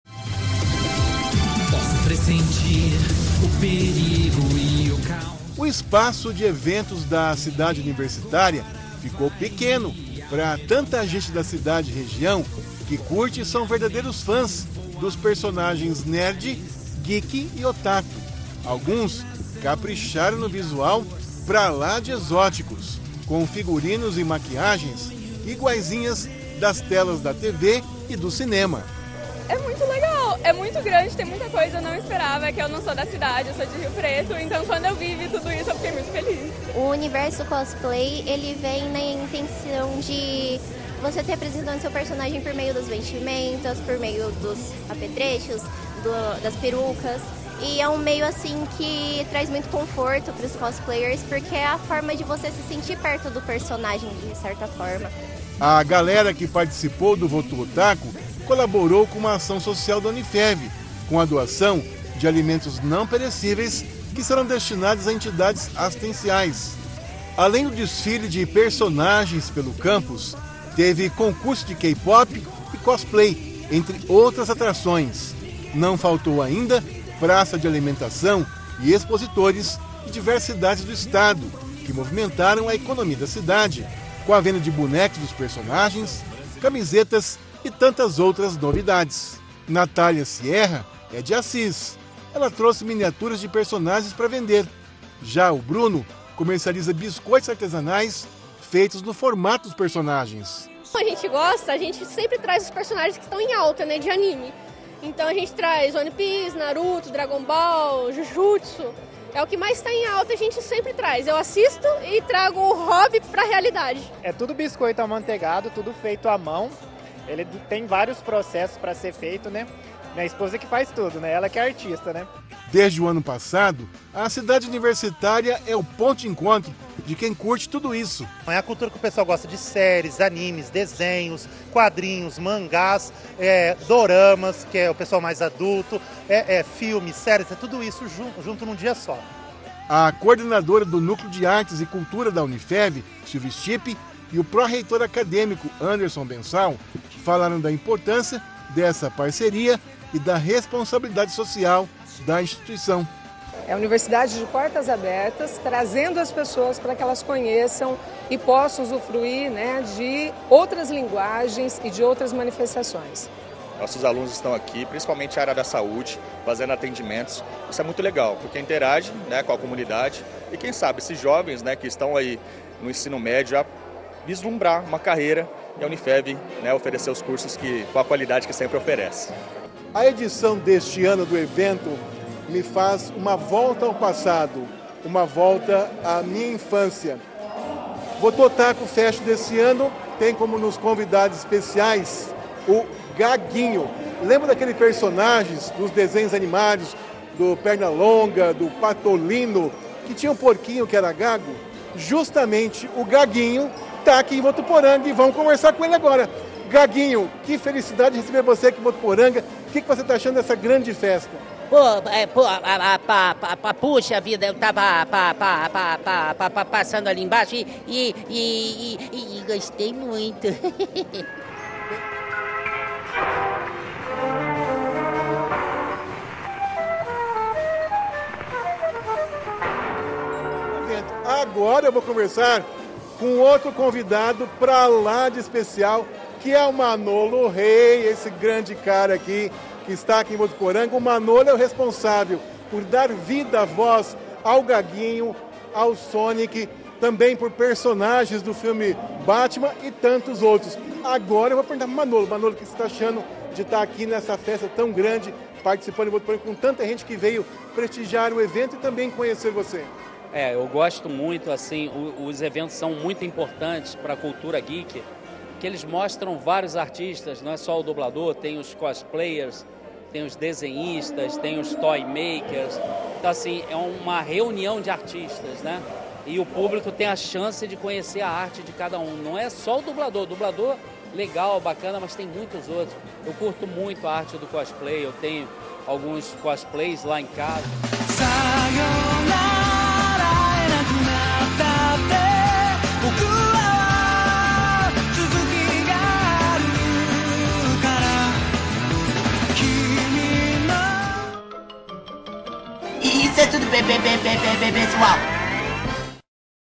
Votu Otaku Fest 2023
A cidade universitária da Unifev sediou no domingo um dos principais eventos de cultura nerd, geek e otaku do noroeste paulista, a Votu Otaku Fest 2023. Entre as atrações mais esperada da festa, foi a presença do dublador Manolo Ray, responsável por dar a vida à diversos personagens de filmes do Homem Aranha, Will Smith na série Um Maluco no Pedaço e do porquinho gago da turma do Pernalonga, Gaguinho.